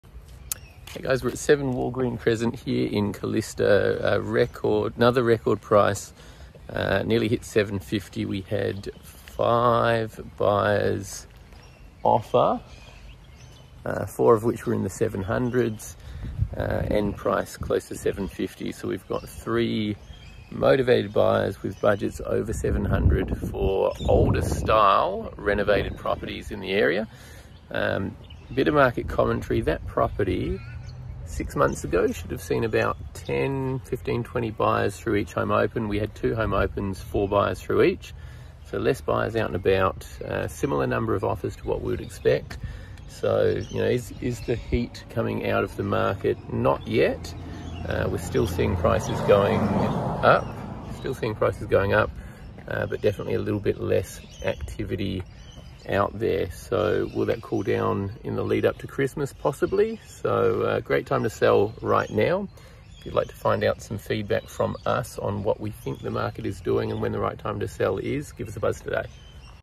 Record Sale Price for Calista! + October property market update from Michael
Wondering how this sale affects the value of your home or simply looking for some insight into the current property market?